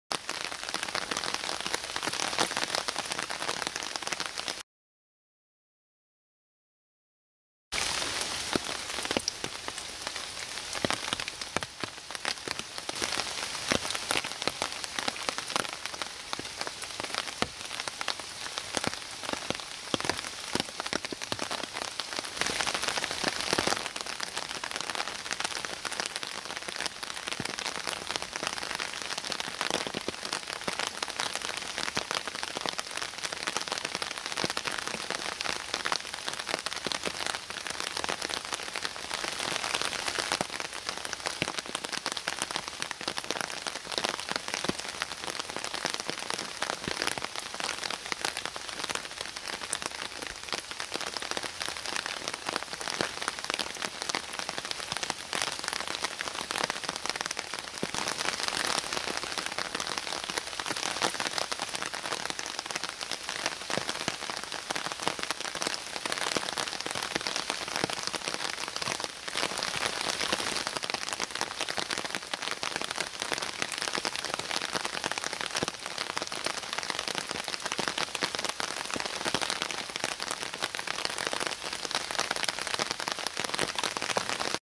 Zwijntjes in de regen
Zomer avond tijdens een regenbui even checken of er nog dieren actief zijn in het bos.